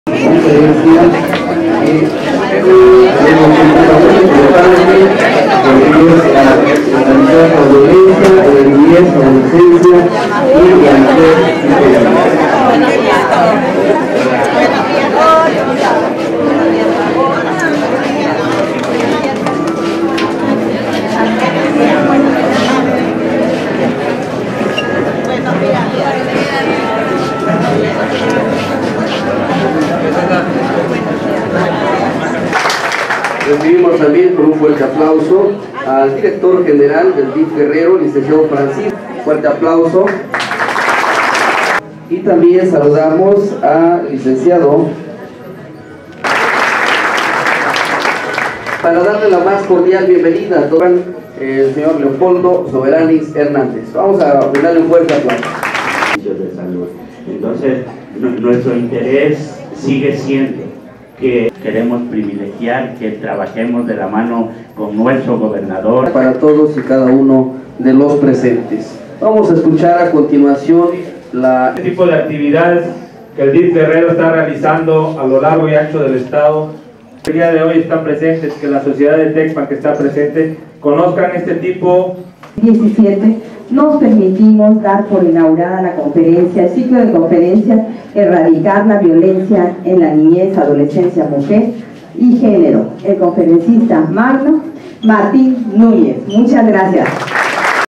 GradoCeroPress GIRA DIF inauguración para erradicar violencia contra la mujer
PONE EN  MARCHA MERCEDES CALVO LA TERCERA SEMANA DE SALUD EN TECPAN

* La presidenta del DIF Guerrero entregó diversos apoyos en Tecpan de Galeana y San Jerónimo

TECPAN DE GALEANA.- Al poner en marcha la Tercera Semana Nacional de Salud en este municipio, la presidenta del Patronato del Sistema DIF Guerrero, Mercedes Calvo de Astudillo, aplicó la primera dosis de un total de 550 mil vacunas que se distribuirán en el estado, donde destacó la importancia de proteger y cuidar la salud de los niños.

Acompañada del secretario de Salud en el estado, Carlos de la Peña Pintos, dieron a conocer que durante la Semana se aplicará la vacuna Sabin contra la polio, la segunda dosis de la vacuna SRP triple viral, vacuna contra el virus del papiloma humano, seguimiento del esquema básico y la vacunación de la influenza, además de ácido fólico.

Durante la gira de trabajo por los municipios de Tecpan de Galeana y San Jerónimo, Mercedes Calvo entregó 200 aparatos funcionales, 260 lentes, 194 servicios del registro civil y a través de las unidades móviles se brindaron 600 servicios como consulta médica, dermatología, odontológica, además de estudios de papanicolaou, mastografía y antígeno prostático.

Más tarde en Tecpan inauguró el ciclo de conferencias: Erradicar la Violencia en la Niñez, Adolescencia, Mujer y Género, donde expresó que orientar e informar a los jóvenes es la mejor estrategia para prevenir y disminuir violencia.

La conferencia fue impartida por Martín Núñez, con el objetivo de que la juventud esté informada y en caso de vivir una situación de violencia tengan los conocimientos de dónde acudir o qué hacer para denunciar este tipo de hechos.

En el mismo evento, entregó el material para equipamiento de la Unidad Básica de Rehabilitación de Tecpan, con una inversión de un millón de pesos, equipada con escalera sueca, espejo postiral, cilindro parafinero, escalera, bicicleta fija y comprecero, lo que permitirá que las personas con problemas físicos puedan rehabilitarse, sin tener que trasladarse al municipio de Acapulco.

En su intervención, el presidente municipal de Tecpan de Galeana, Leopoldo Soberanis Hernández, agradeció el proyecto del Hospital Básico Comunitario y destacó “les expresamos nuestra gratitud y reconocimiento porque con esto mejoraremos los servicios de salud que le ofrecemos a la población y reconocemos el trabajo del gobernador Héctor Astudillo para que Guerrero cambie y salga adelante”.

Por último puso en marcha la campaña de donación de lentes en donde se entregarán mil 800 en diversos municipios del estado, además de que entregó mochilas con paquetes escolares y apoyo alimentario en ambos municipios de la Costa Grande.

La acompañaron el director general del DIF Guerrero, Francisco Solís Solís, el presidente municipal de Atoyac, Dámaso Pérez y la alcaldesa de San Jerónimo, Deysi Coria.